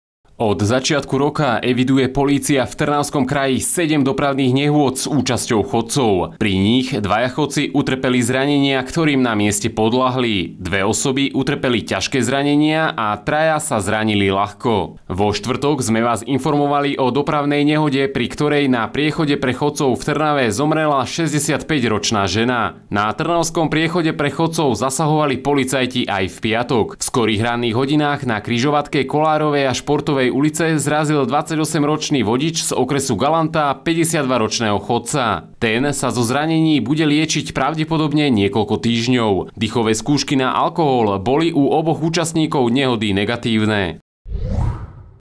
ĎALŠIA NEHODA NA PRIECHODE
Od začiatku roka eviduje polícia v Trnavskom kraji 7 dopravných nehôd s účasťou chodcov.